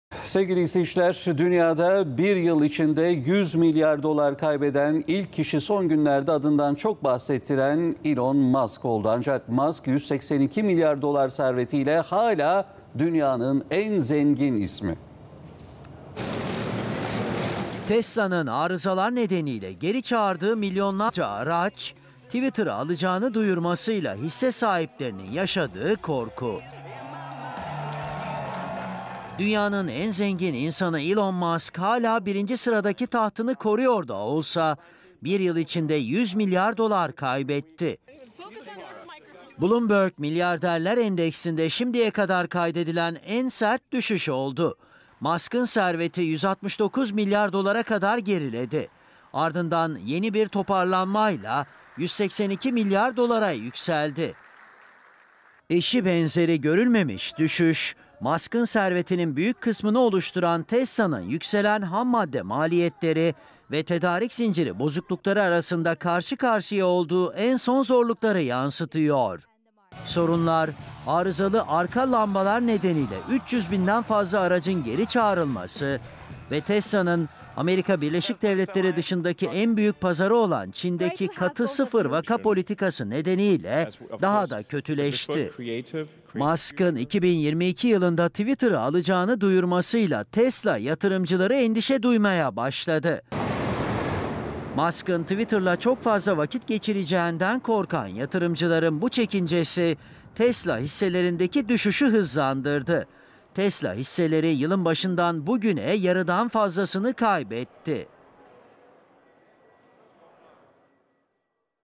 Elon Musk 100 milyar dolar kaybetti!
Twitter'ı satın alması ve şirkette yarattığı çalışan kıyımıyla gündemin ilk sıralarında yer alan Elon Musk'ın serveti erimeye devam ediyor. Musk'ın servetinde bu yıl yaşanan gerileme ilk kez 100 milyar doları aştı. Milyarder iş insanı sert düşüşe rağmen 'dünyanın en zengin insanı' unvanını korumayı başardı.